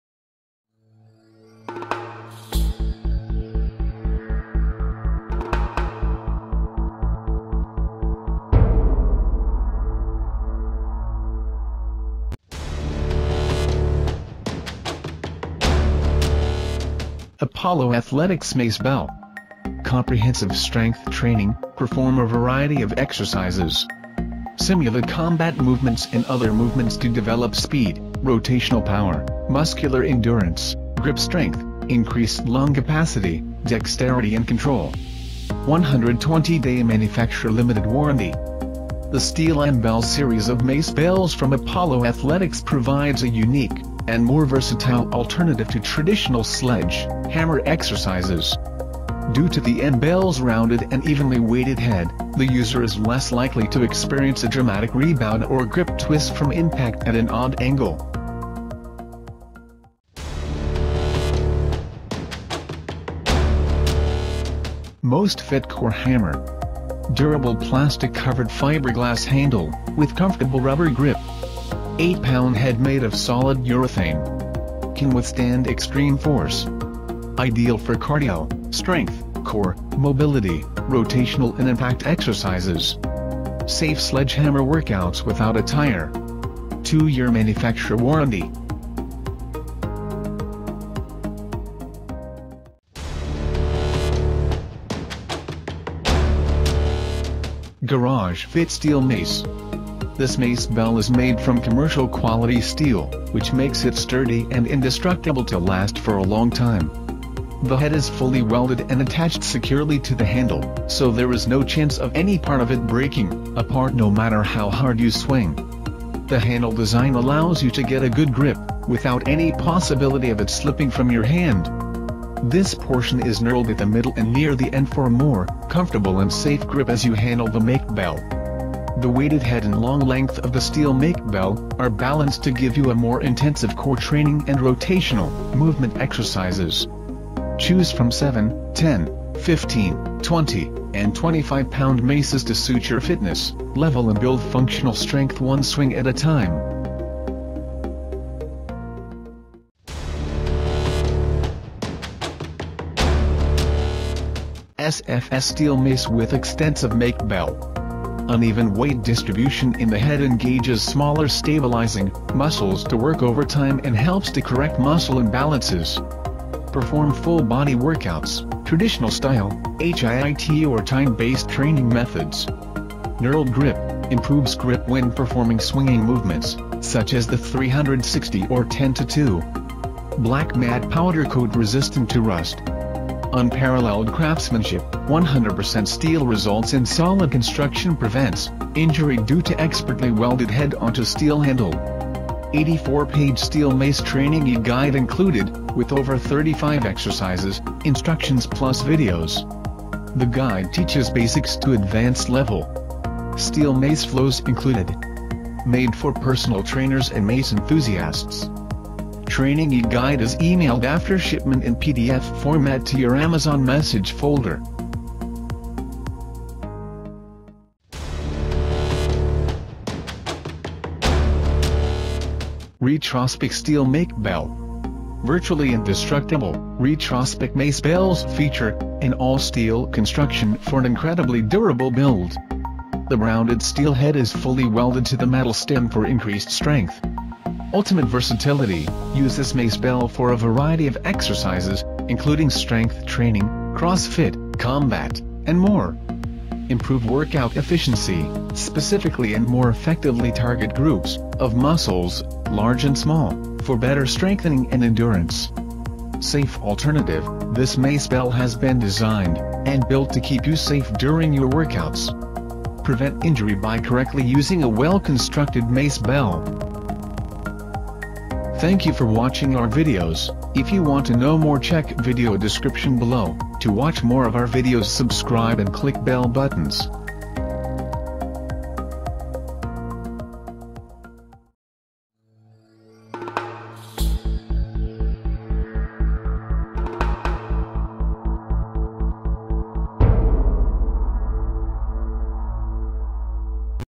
Macebells - The Best Macebells Reviews 2020
The Best Macebells Suitable for preparing at home or in the rec center. Agreeable under the length and thickness of the handle with two territories for grasp doesn't sneak out of the hands because of scores

Here Are The Top 5 Macebells :

1.Apollo Athletics Mace Bell

2.MostFit Core Hammer

3.Garage Fit Steel Mace

4.SFS Steel Mace with Extensive Macebell

5.Retrospec Steel Macebell